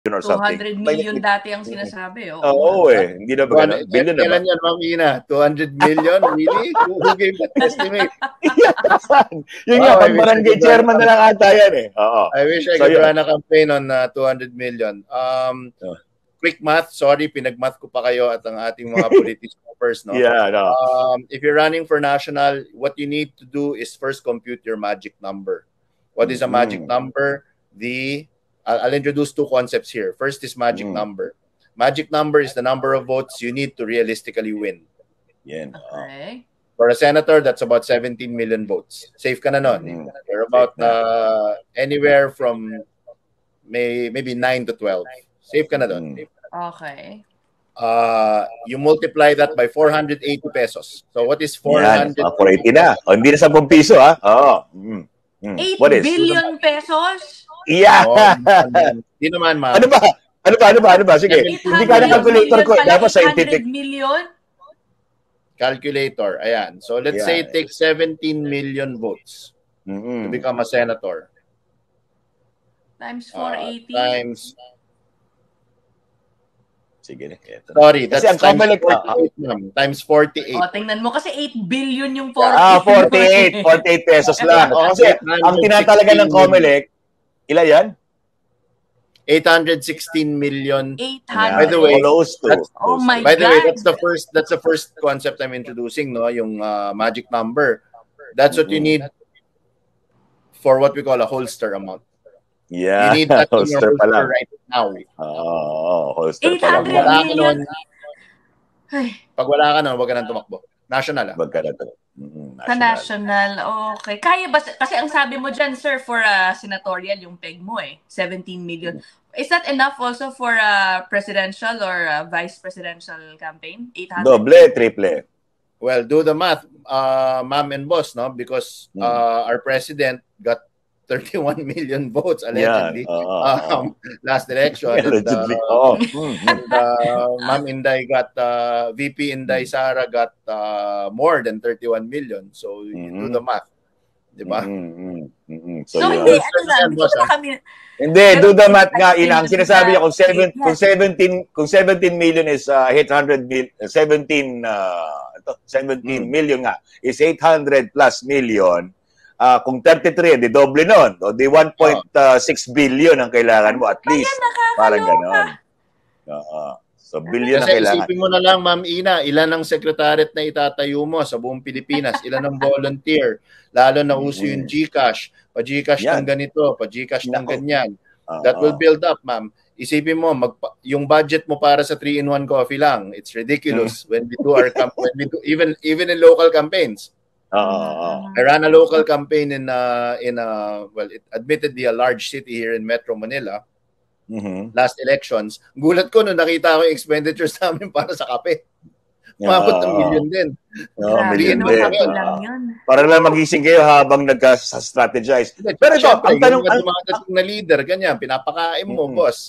P800M minimum budget para sa national campaign!
Ayon kay campaign strategist Alan German, kung walang P800M ang tatakbo sa national elections ay hindi na dapat ito tumuloy.